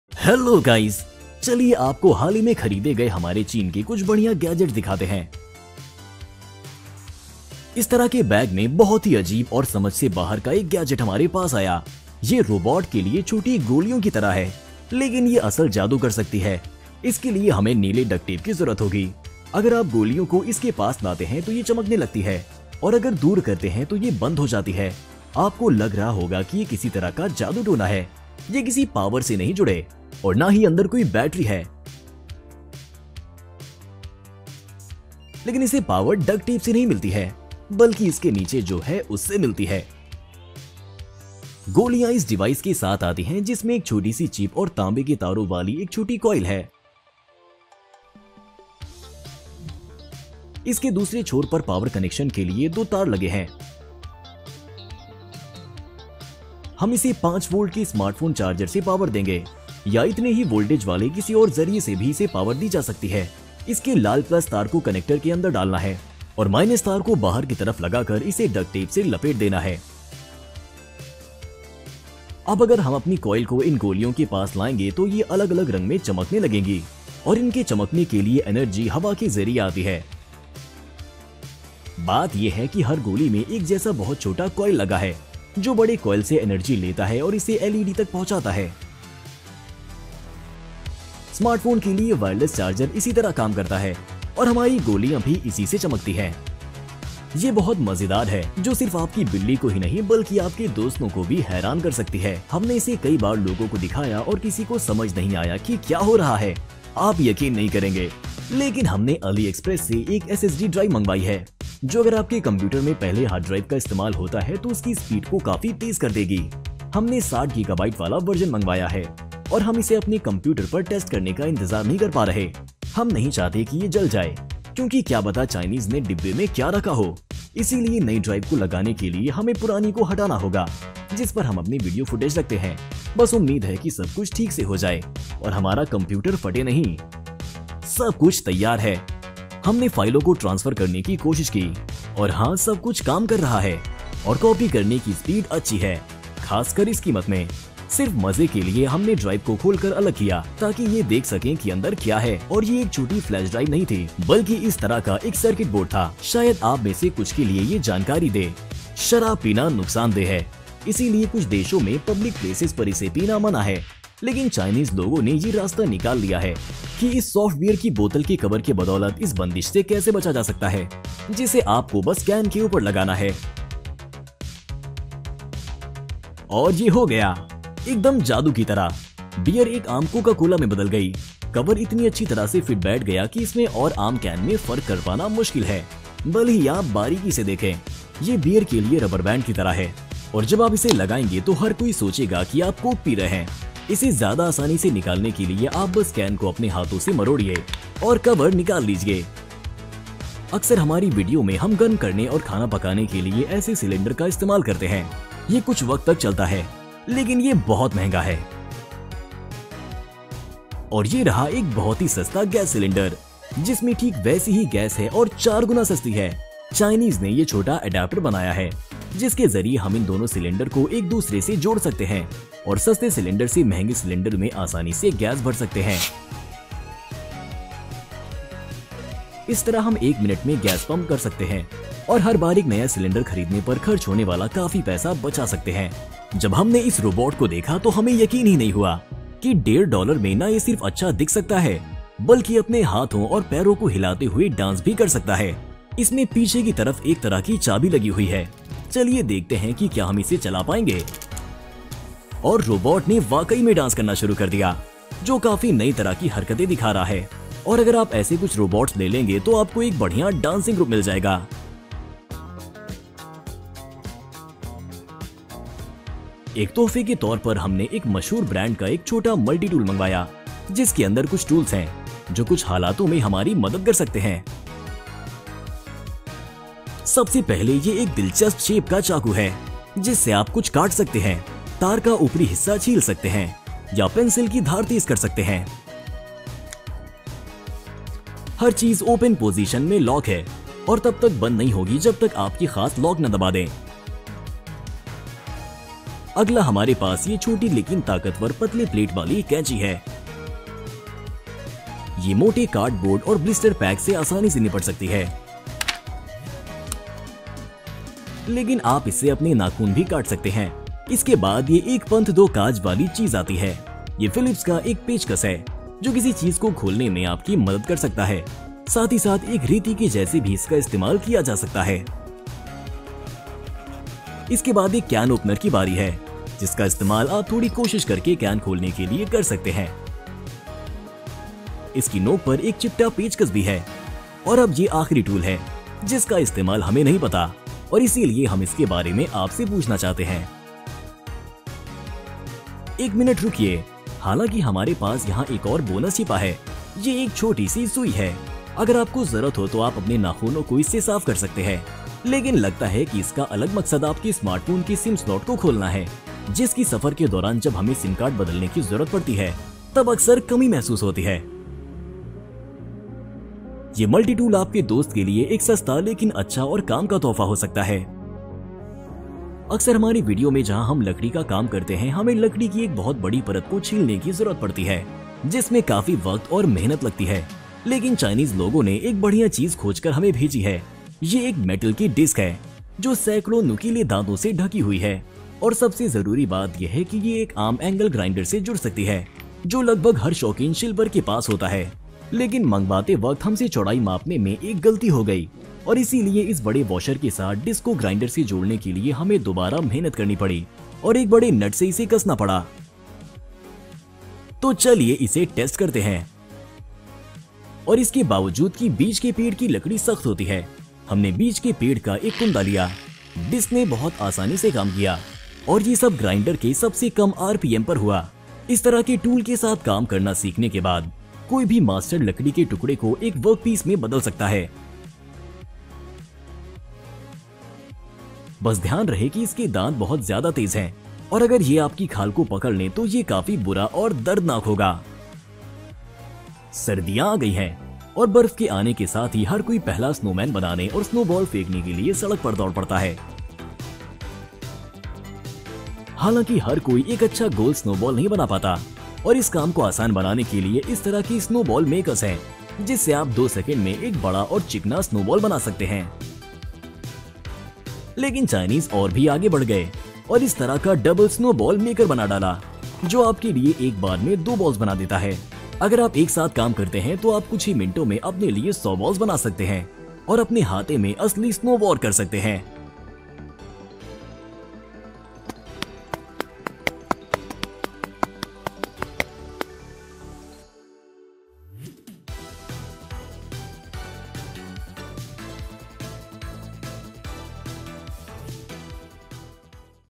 हमने इसे अलीएक्स्प्रेस से खरीदा
लकड़ी छीलने वाला पहिया (एक स्टैण्डर्ड ग्राइंडर के लिए 22 mm वाला लें)